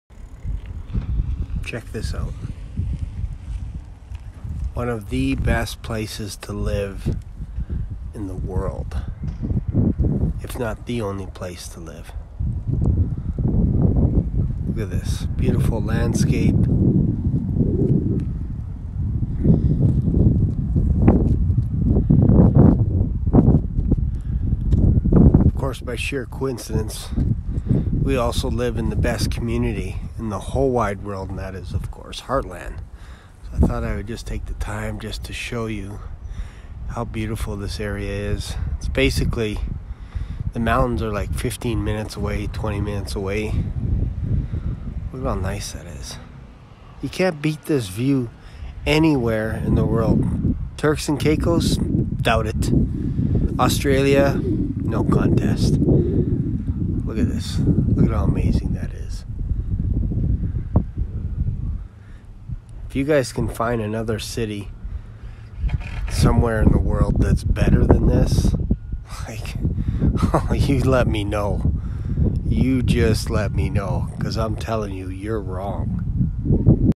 THE BEST PLACE TO LIVE IN THE ALBERTA | CANADA
I have compiled a tonne of research and the stats back it up.

Cochrane is one of the best places to live in the world.

Not just the top ten in all of Canada but the top place to live in the world.

I also happen to live in the best community in the world as well. Heartland, Cochrane Alberta.

Coincidence ? No my friends...Science.

Look at the view? This is the best view in the world. So when you move to Cochrane you get to have three things.

The worlds best city.
The worlds best community.
The worlds best view.

Amazing isn’t it?

I think national geographic should put Cochrane in the Center of its latest addition. Kinda like a Center fold but not that kind.

Am I crazy? Maybe. Is Cochrane that damn nice? 100%

If you decide to move to Cochrane and you don’t think this is the nicest place on Earth I will personally ask your realtor to give you your money back. With interest.

What you think boring Turks and Caicos is better? Oh you think Australia is nicer? Please in your dreams.